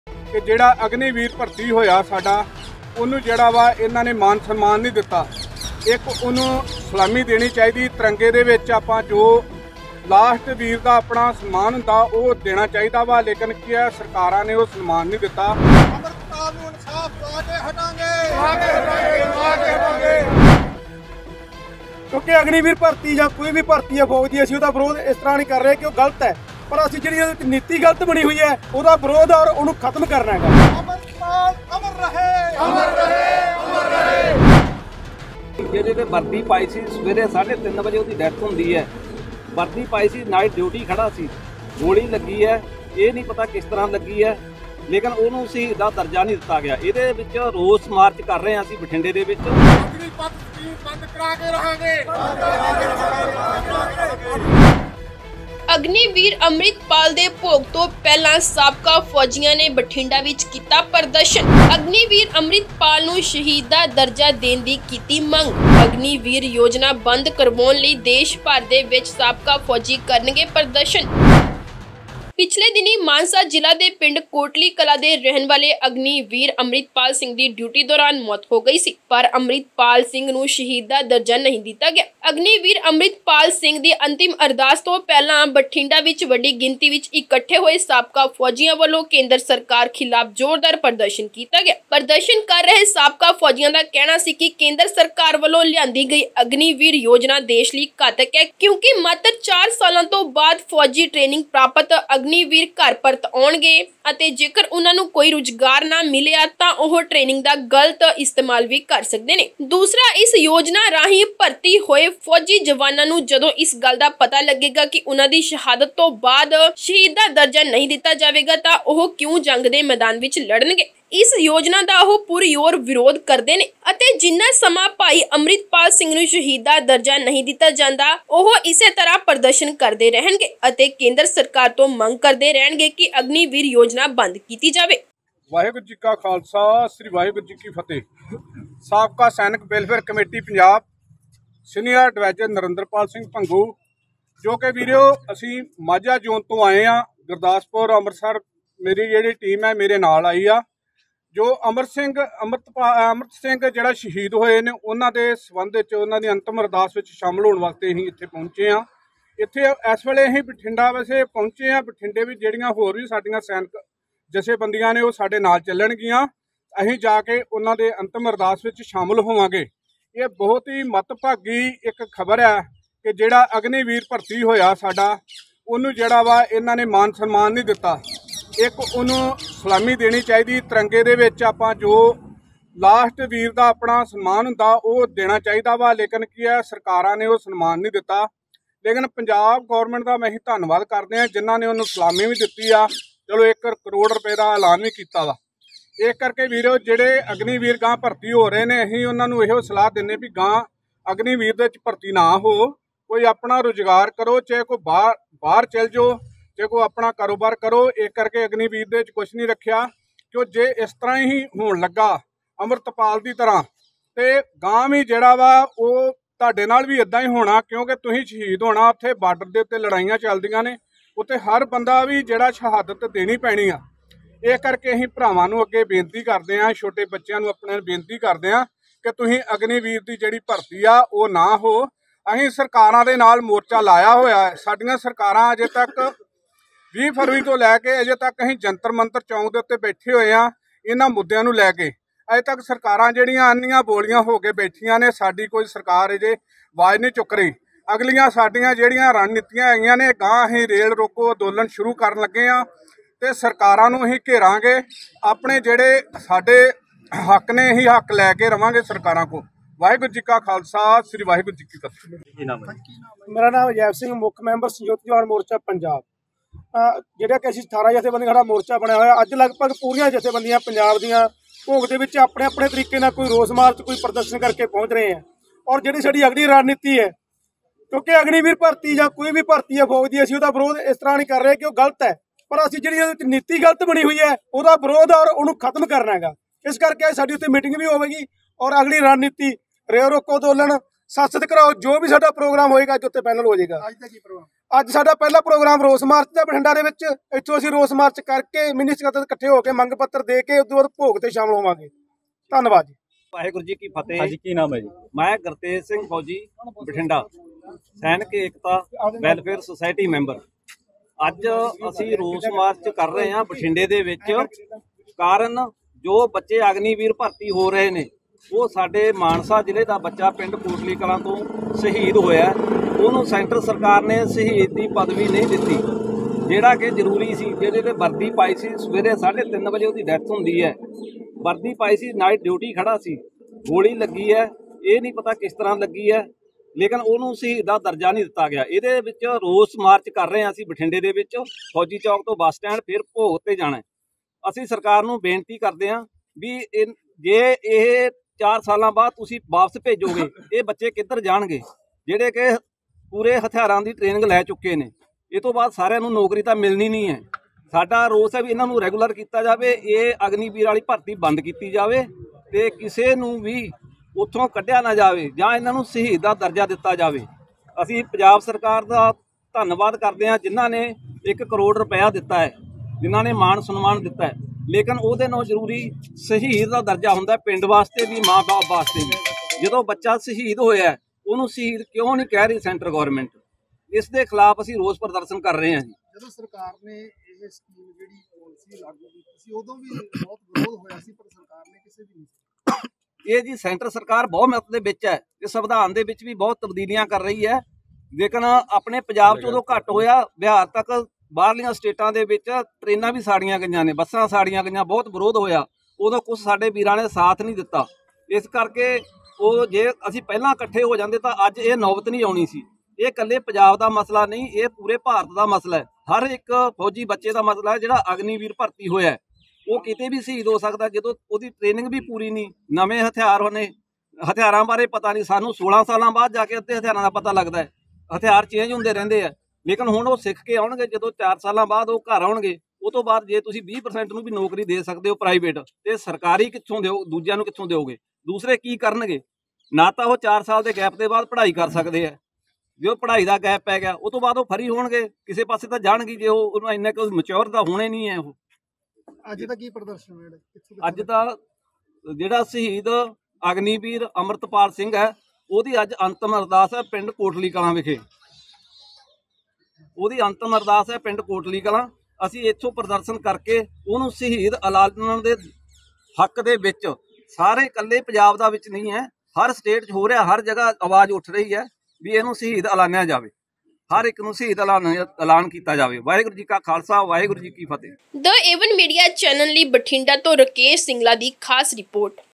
BATHINDA ਅਗਨੀਵੀਰ ਅੰਮ੍ਰਿਤਪਾਲ ਦੇ ਭੋਗ ਤੋਂ ਪਹਿਲਾਂ ਸਾਬਕਾ ਫੌਜੀਆਂ ਨੇ ਕੀਤਾ ਪ੍ਰਦਰਸ਼ਨ
BATHINDA ਅਗਨੀਵੀਰ ਅੰਮ੍ਰਿਤਪਾਲ ਦੇ ਭੋਗ ਤੋਂ ਪਹਿਲਾਂ ਸਾਬਕਾ ਫੌਜੀਆਂ ਨੇ ਕੀਤਾ ਪ੍ਰਦਰਸ਼ਨ, ਅਗਨੀਵੀਰ ਅੰਮ੍ਰਿਤਪਾਲ ਨੂੰ ਸ਼ਹੀਦ ਦਾ ਦਰਜਾ ਦੇਣ ਦੀ ਕੀਤੀ ਮੰਗ, ਅਗਨੀਵੀਰ ਯੋਜਨਾ ਬੰਦ ਕਰਵਾਉਣ ਲਈ ਦੇਸ਼ ਭਰ ਚ ਸਾਬਕਾ ਫੌਜੀ ਕਰਨਗੇ ਪ੍ਰਦਰਸ਼ਨ , ਕੇਂਦਰ ਸਰਕਾਰ ਅਤੇ ਪੰਜਾਬ ਸਰਕਾਰ ਖਿਲਾਫ ਕੀਤੀ ਜ਼ੋਰਦਾਰ ਨਾਅਰੇਬਾਜ਼ੀ |